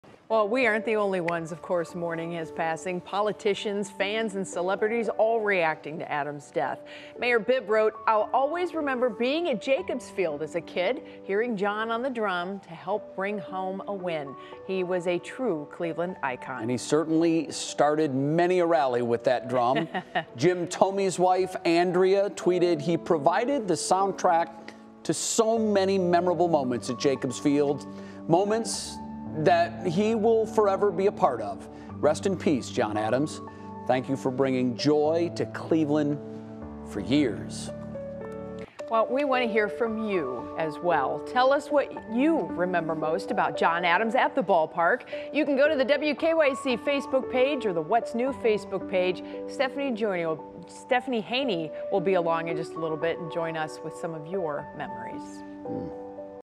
Fans, politicians, celebrities mourn death of iconic Cleveland baseball drummer John Adams
Cleveland baseball fans are in mourning, with the Guardians announcing Monday that iconic fan John Adams has died at the age of 71.

For more than 40 years, Adams banged his large drum from the bleachers at both Cleveland Municipal Stadium and later Jacobs/Progressive Field, rarely missing games as he cheered on the then Indians through some of their darkest days but also brightest moments. He was one of the few constants through decades of change both in baseball and around Northeast Ohio, helping create countless memories for millions of people at the ballpark.

Once news of John's passing became official, many of those people took to social media to express their sadness and pay tribute the city's No. 1 baseball fan, with politicians and celebrities among those looking back on what made Adams and his drum so special.